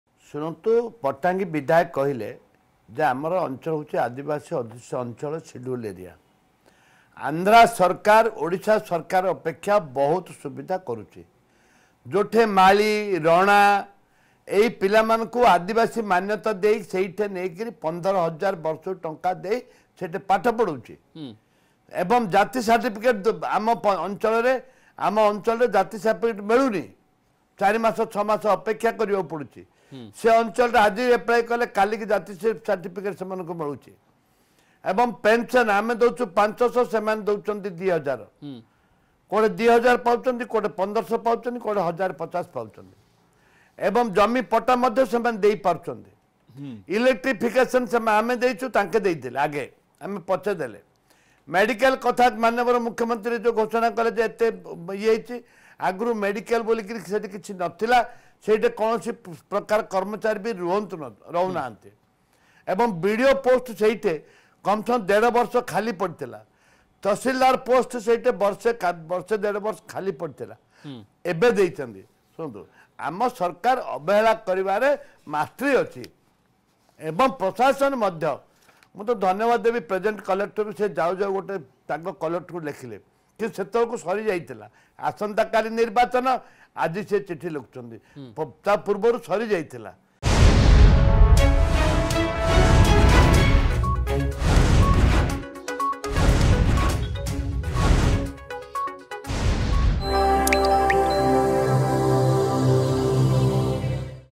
ଆନ୍ଧ୍ର ସରକାର ଓଡିଶା ସରକାରଙ୍କ ଅପେକ୍ଷା ଭଲ ସୁବିଧା ଦେଇଛି..
ଆନ୍ଧ୍ର ସରକାର ଓଡିଶା ସରକାରଙ୍କ ଅପେକ୍ଷା ଭଲ ସୁବିଧା ଦେଇଛି...କୋଟିଆ ନିର୍ବାଚନରେ ଓଡିଶା ଲୋକମାନଙ୍କ ଭୋଟଦାନ ପ୍ରସଙ୍ଗରେ ଏମିତି କହିଲେ କଂଗ୍ରେସ ନେତା ତାରା ବାହୀନି ପତି.